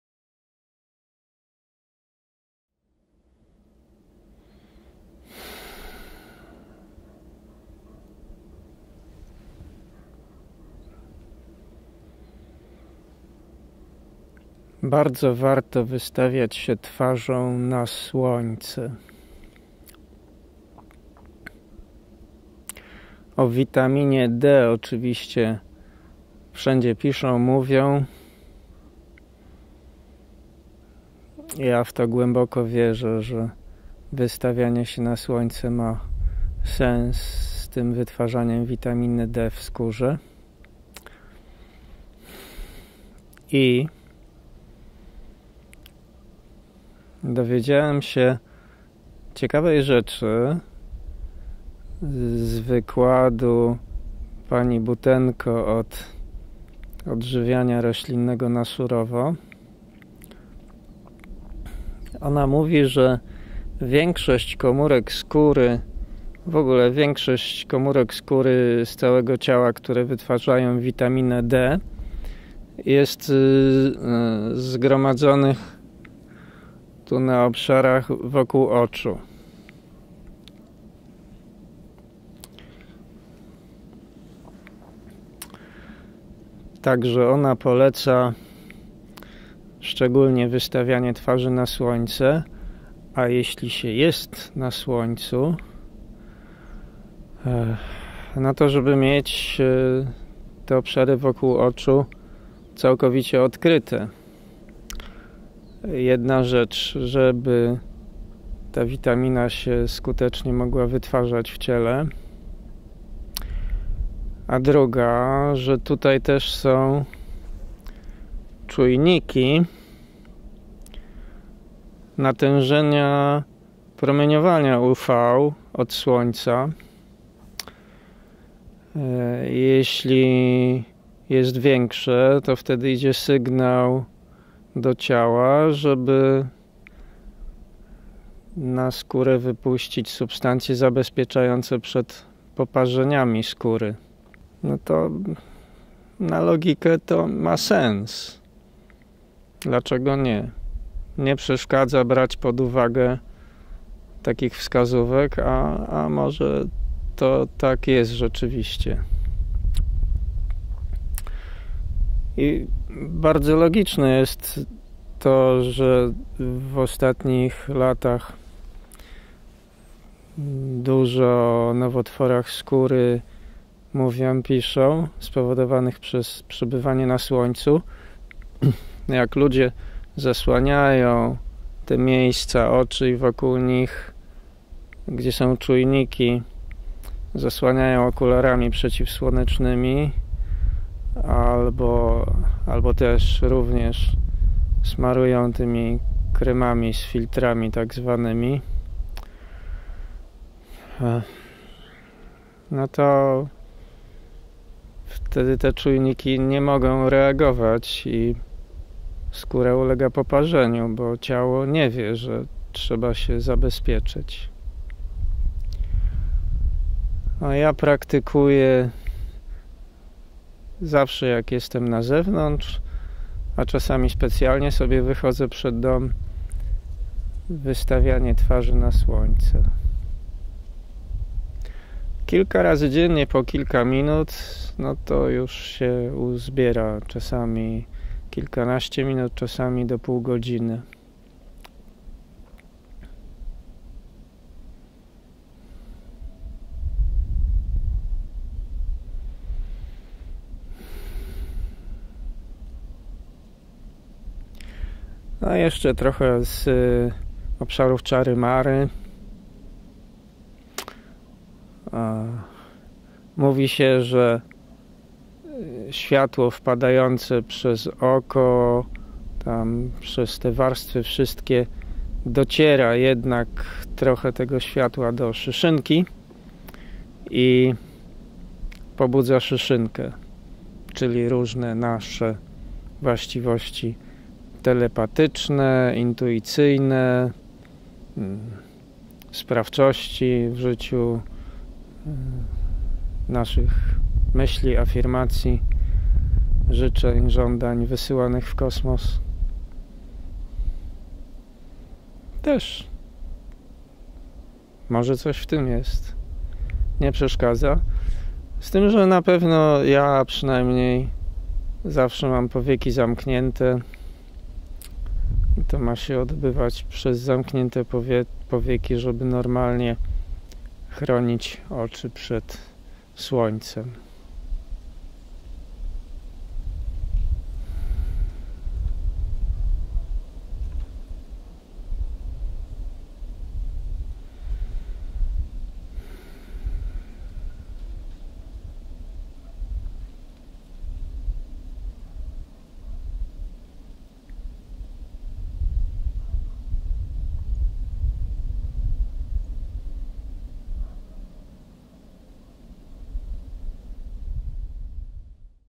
Twarzą do Słońca i witamina D
Warte do wziecia pod uwagę wyjaśnienia na temat wytwarzania witaminy D w skórze od wystawiania twarzy od Słońce